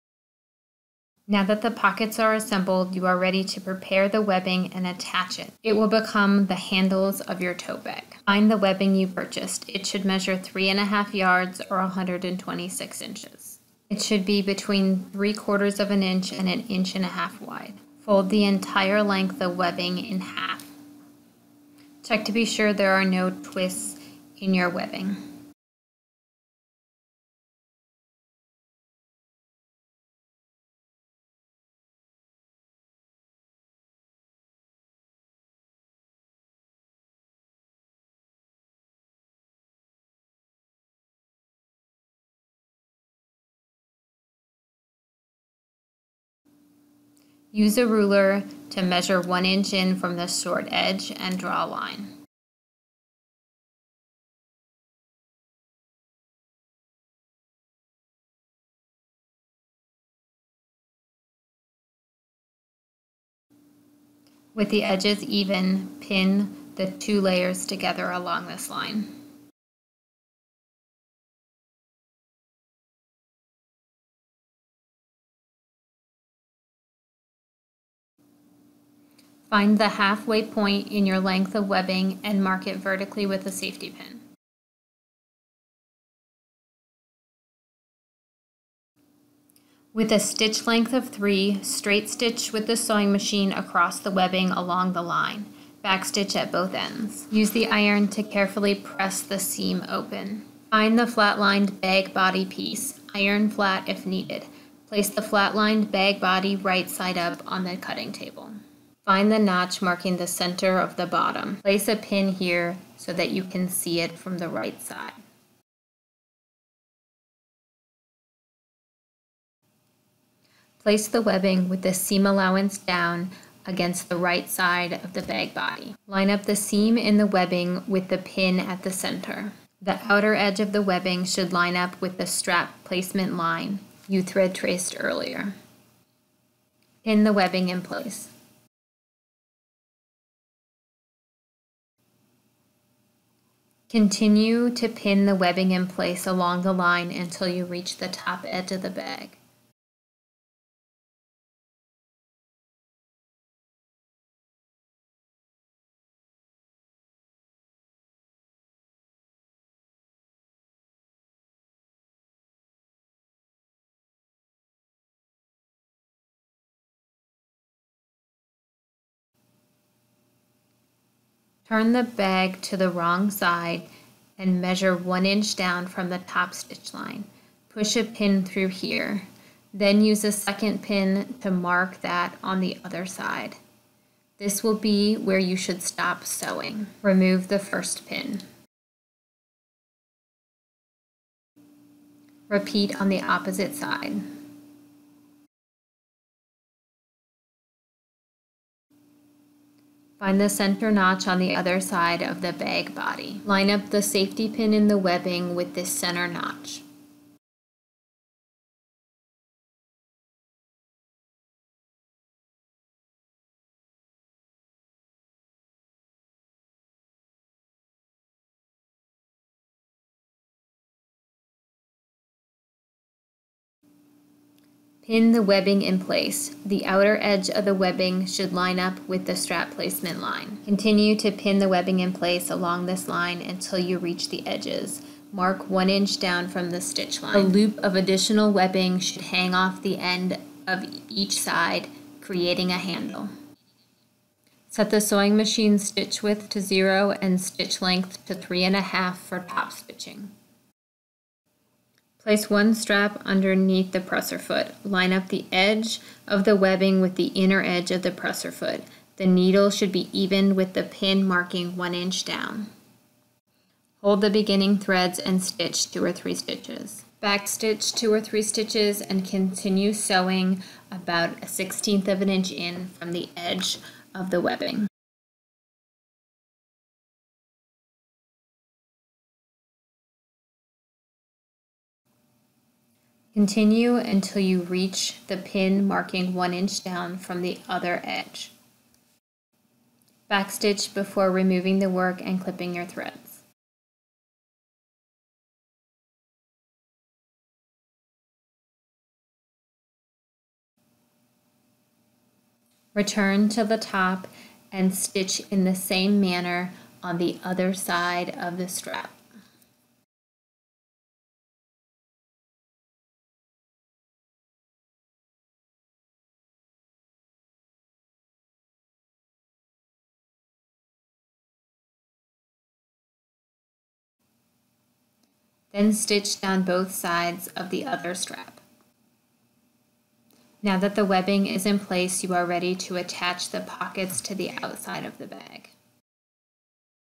Project 3.8 Straps
Instructions for Theatre 40 Lab Project 3 for assembling and attaching the straps to the bag body.